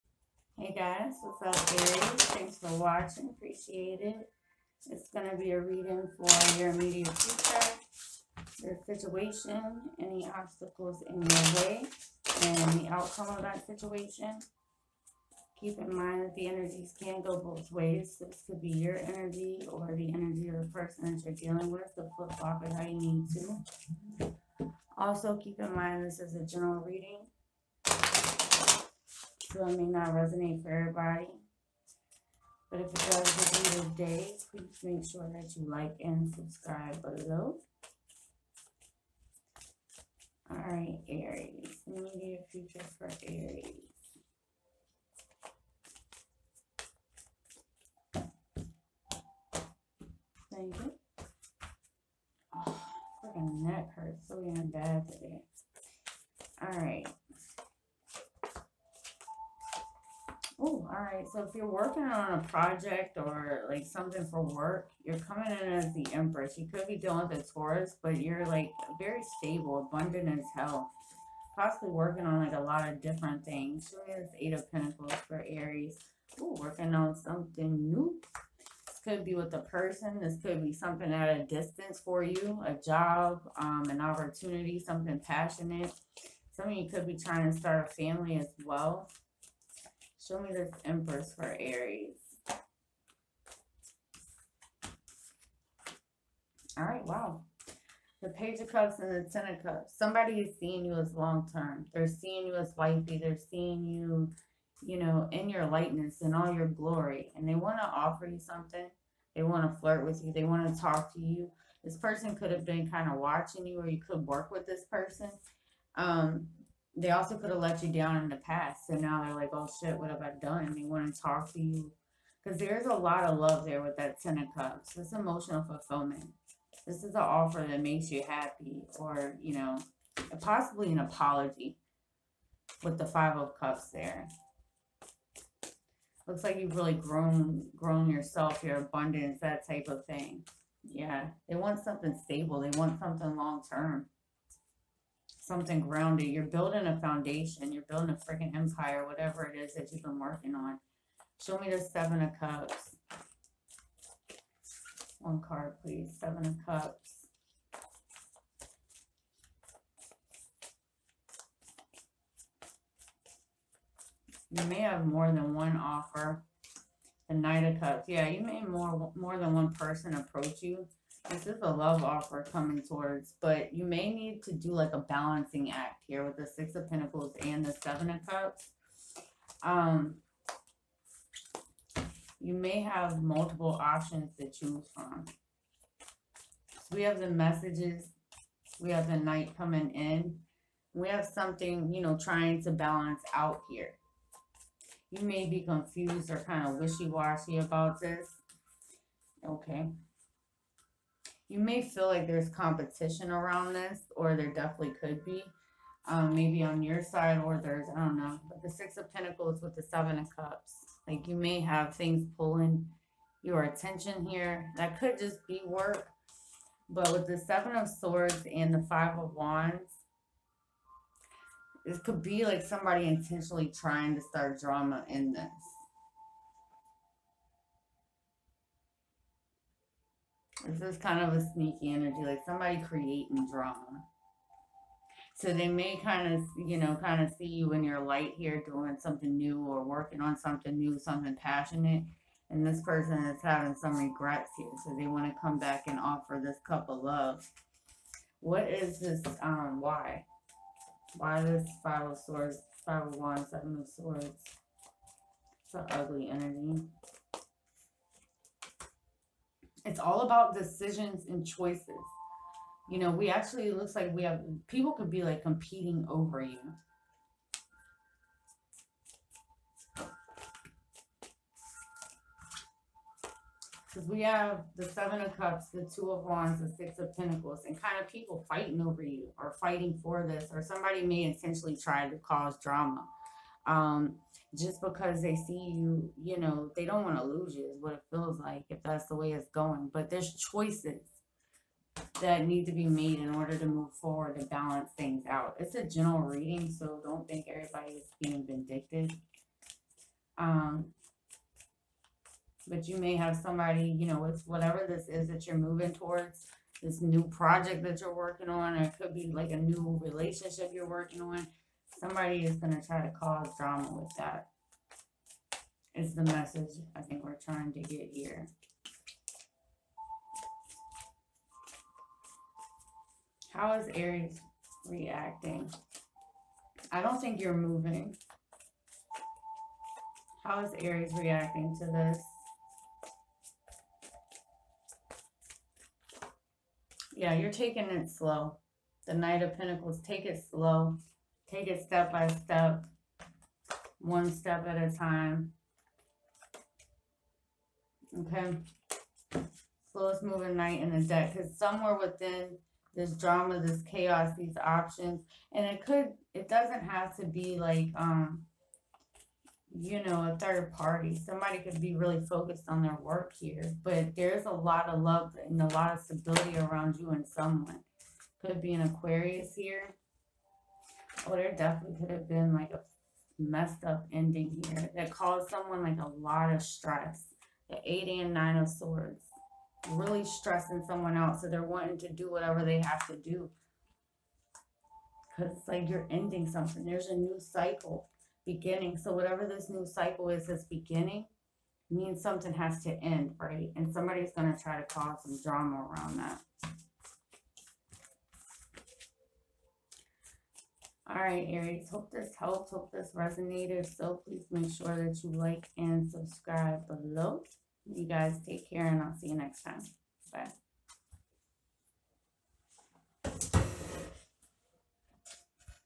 Aries Love Tarot....Balance And Maintain....Immediate Future
Decks used:
Gilded Tarot: Ciro Marchetti
Everyday Tarot
The Light Seer's: Chris-Anne